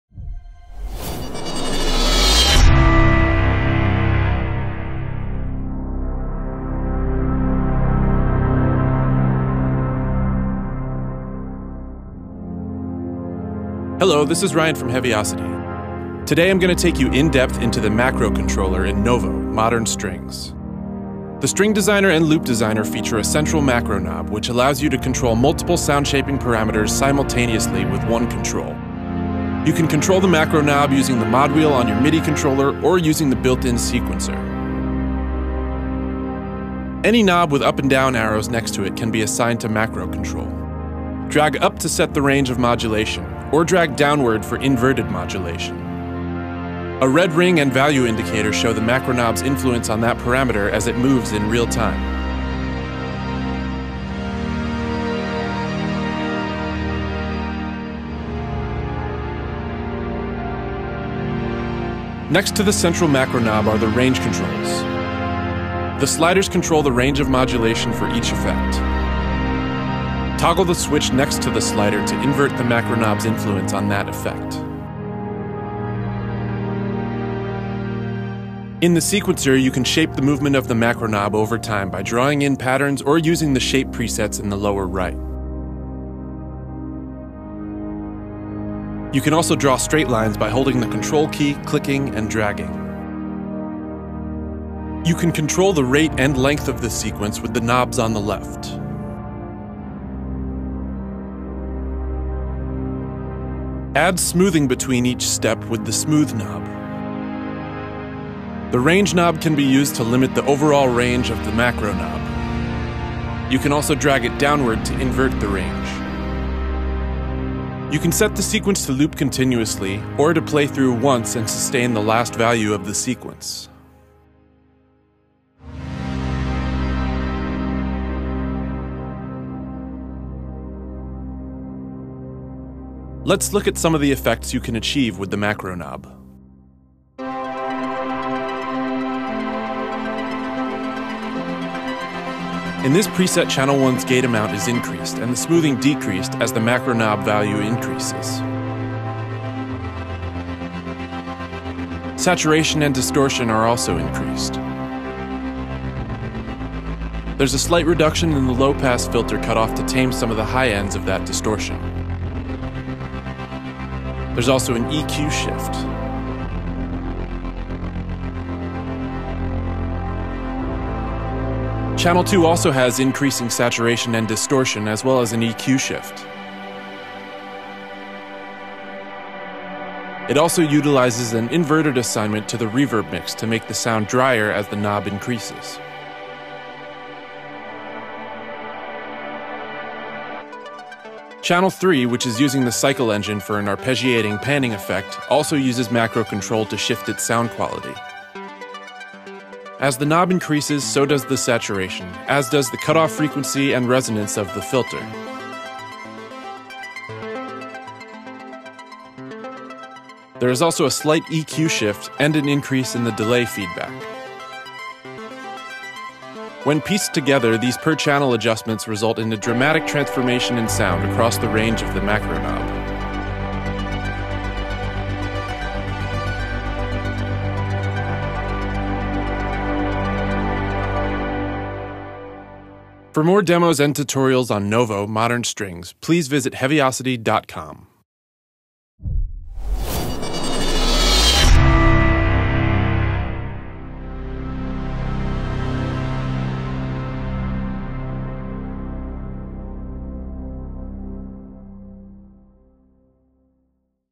NOVO - Macro Control Tutorial | Heavyocity
The Macro Control in NOVO's Hybrid String Designer is a powerful tool for quickly building compelling cinematic textures and elements. Watch Ryan Mallen walk you through this dynamic new feature in this tutorial.

Video Contents:
0:00 – Intro
0:37 – Assigning to the Macro Control
1:02 – Range Controls
1:18 – Macro Sequencer
2:13 – Macro Knob Effects
4:02 – Subscribe!